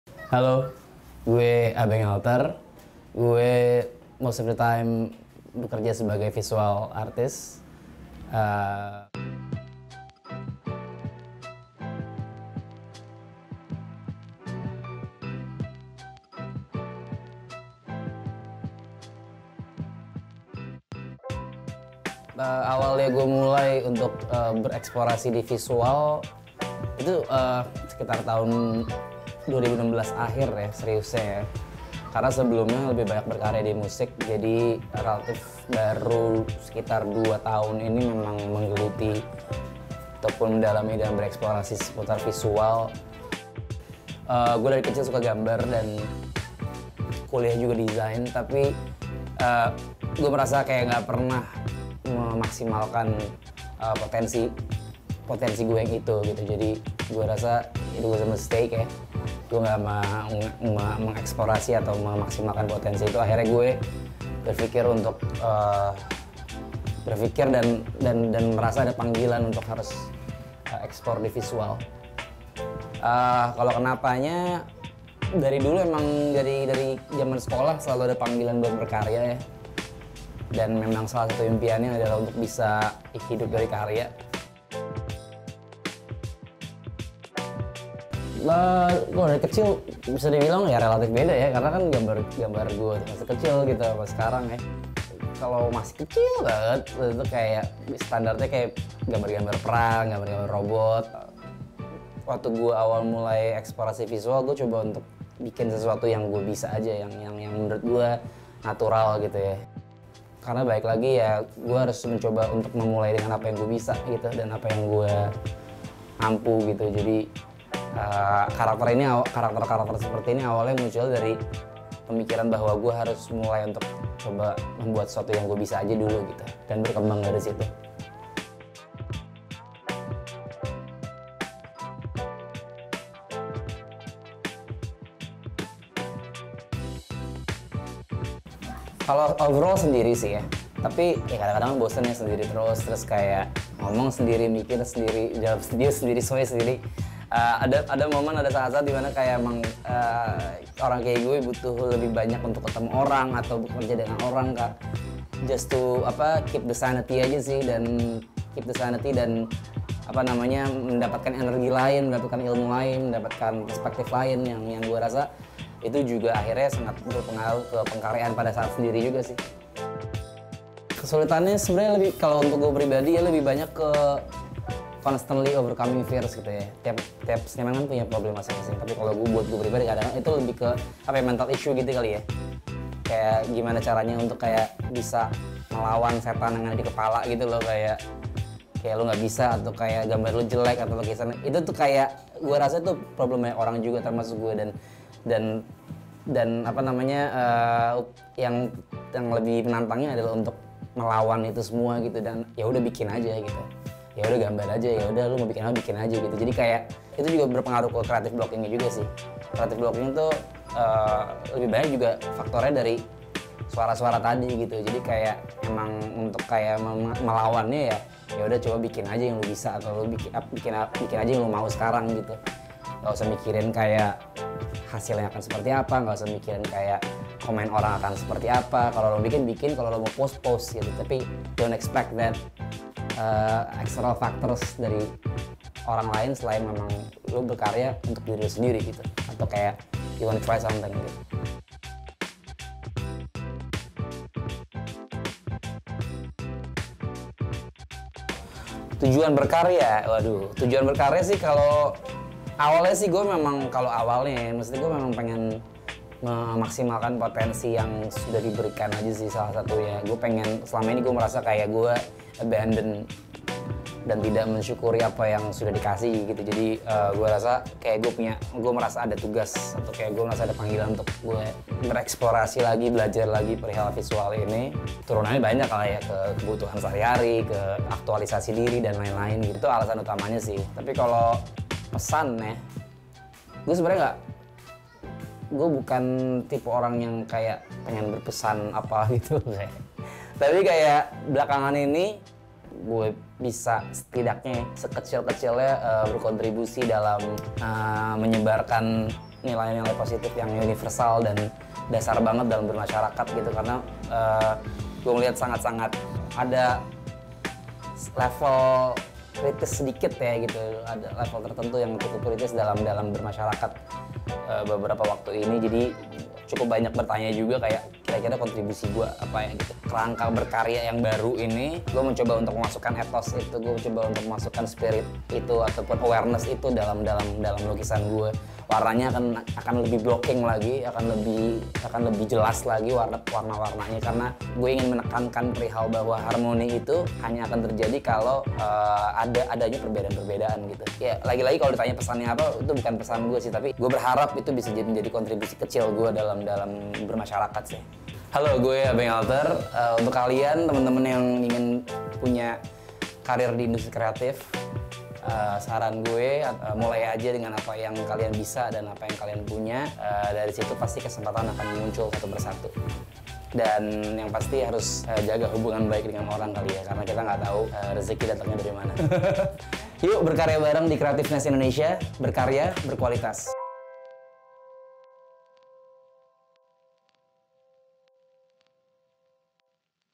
Abenk Alter, Visual Artist | GENERASI JAMAN NOW
Sebagai seorang Visual Artist, creative blocking seringkali datang dari diri sendiri. Hal ini dialami oleh Abenk Alter yang merasa mengalami mental issue sehingga kadang hal itu menghambat proses dalam berkarya. Gimana cara Abenk mengatasi creative blocking yang dia alami? Yuk kita simak cerita Abenk di Generasi Jaman Now kali ini!

*) Footage Credits: Instagram @abenkalter & @abenkalter_space